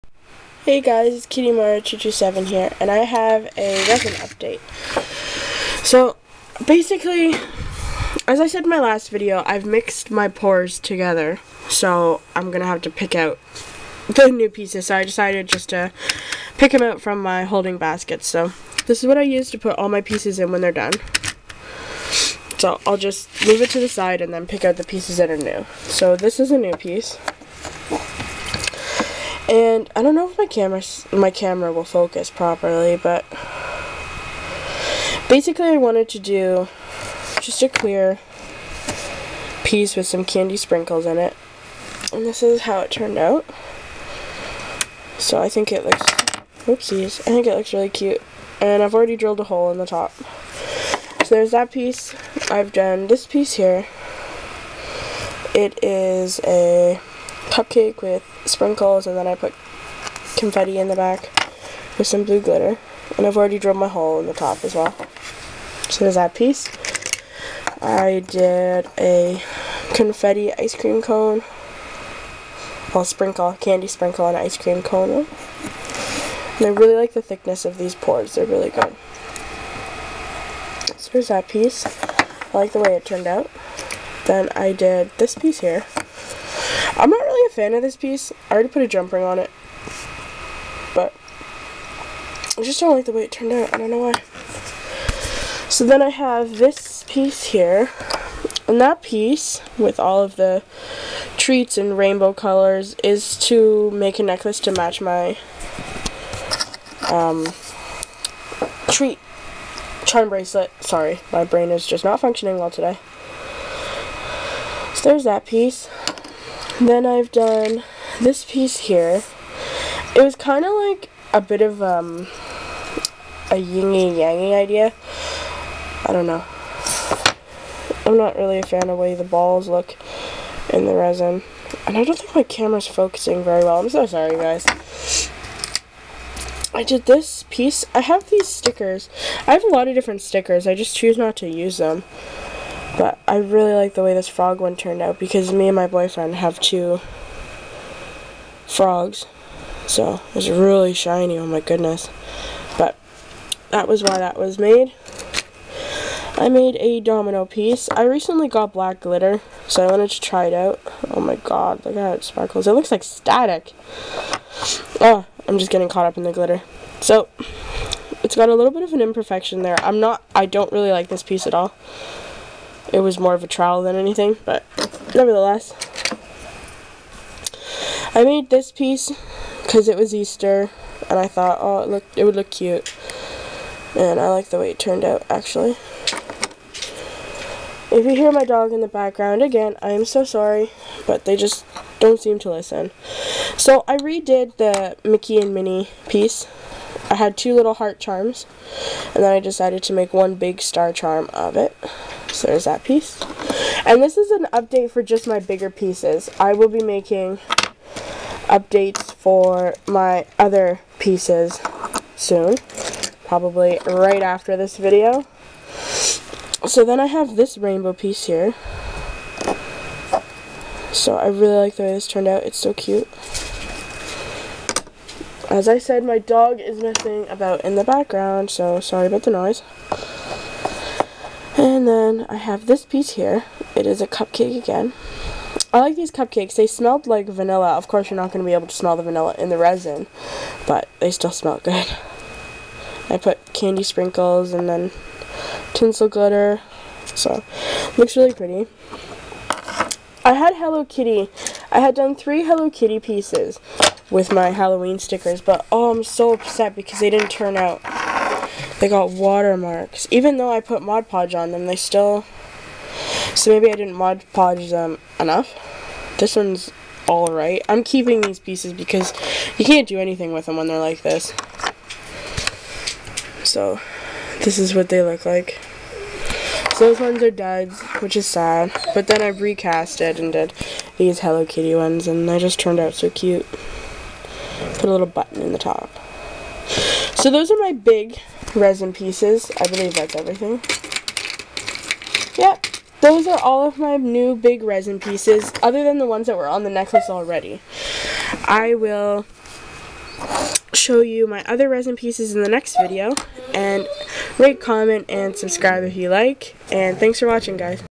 Resin Reveal #7
I wanted to show off the new resin pieces I made!